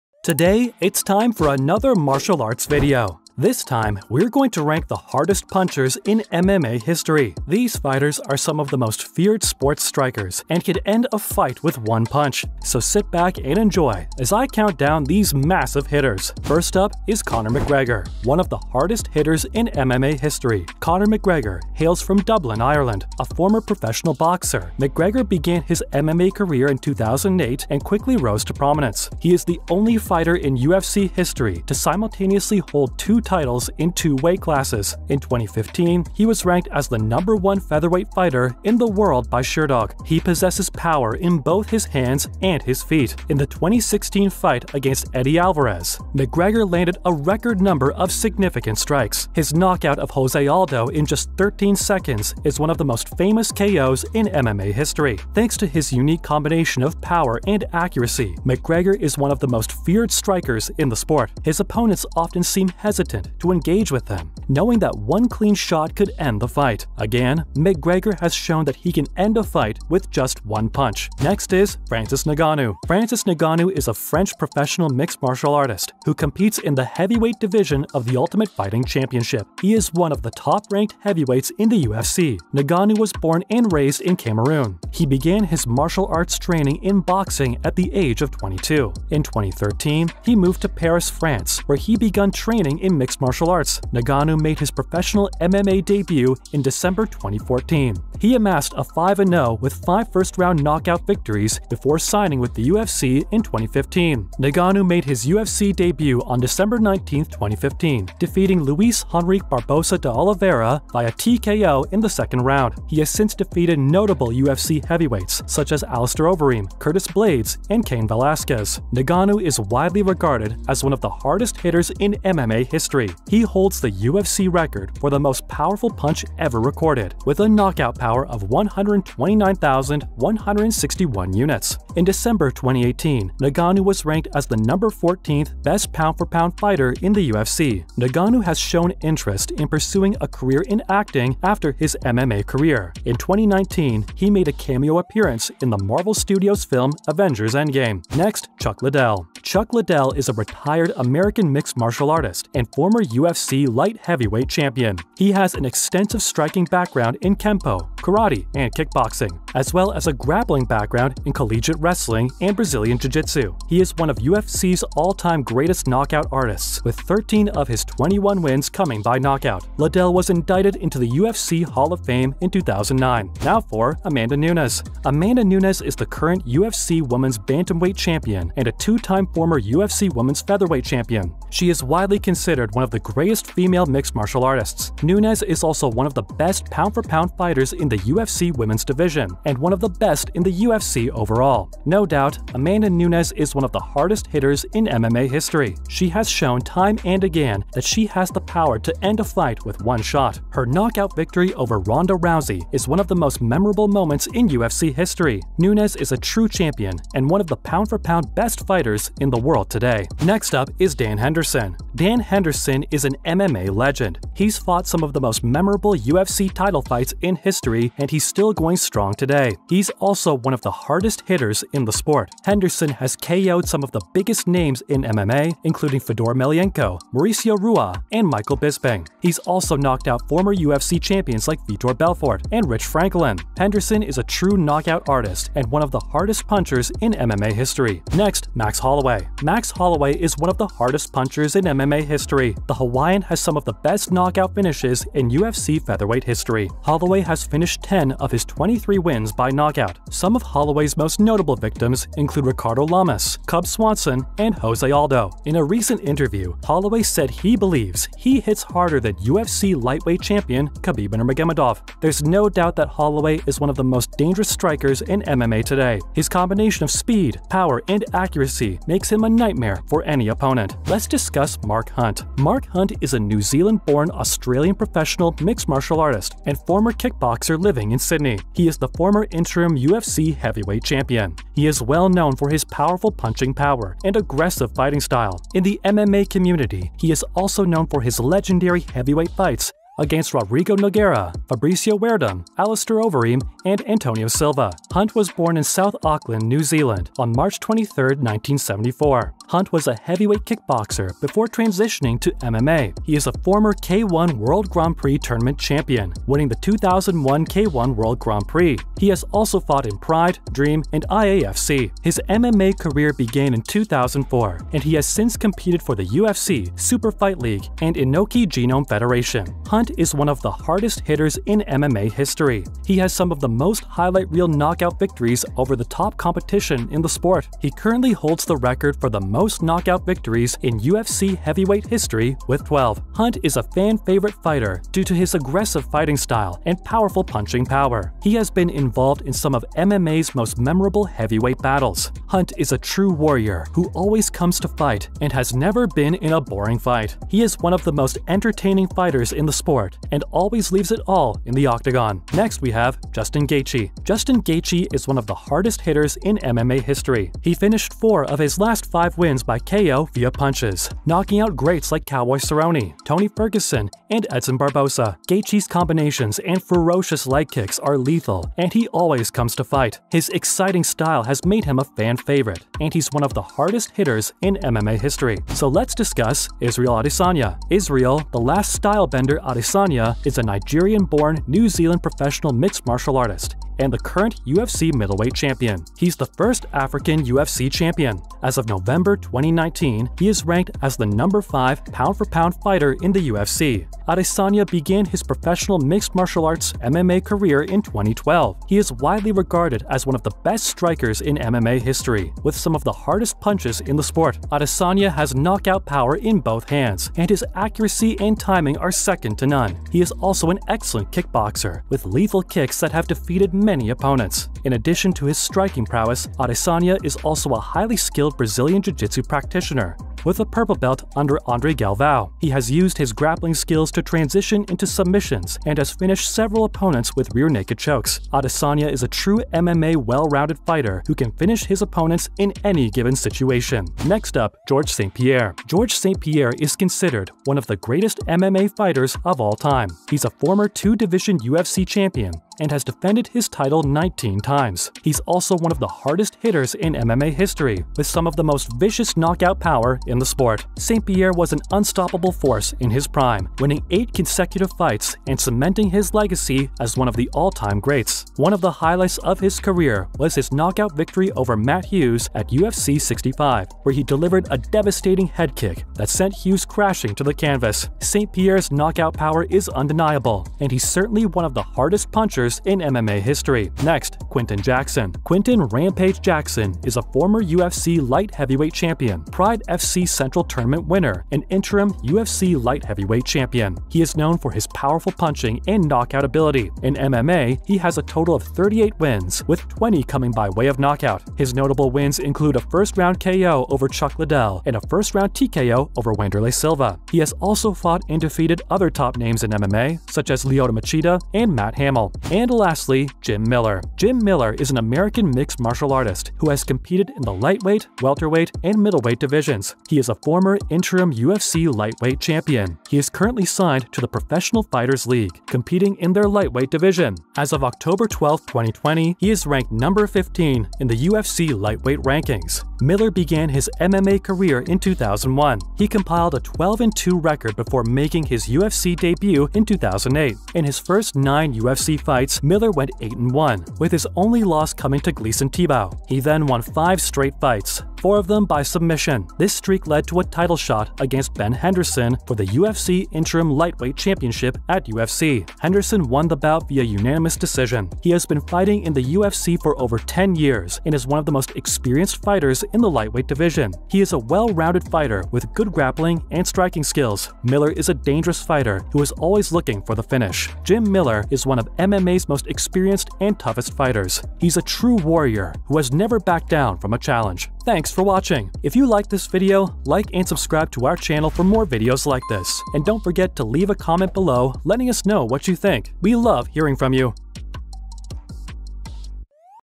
MMA's Hardest Punchers Ranked..
Welcome back to Fighters Hype, today on this channel we are going to talk about “ Hardest Punchers In MMA History, Ranked” The sport of MMA is littered with plenty of hard punchers. However, one stands above the rest in heavyweight fighter Francis Ngannou. Who was registered by the UFC as the hardest puncher on record. Measuring 96 horsepower, which is equal to being hit by a Ford Escort going at full speed.

Known for knocking out some of the biggest names in Pride fighting championships. The Axe Murderer Wanderlei Silva is regarded as one of the hardest hitters in the sport. And this can be attested to by some of his former opponents who have rated him as one of the strongest punches with which they have ever been hit. And when you look at some of the people he has fought, we can see the huge disparities in weight he had to overcome. Check this video out and stay updated!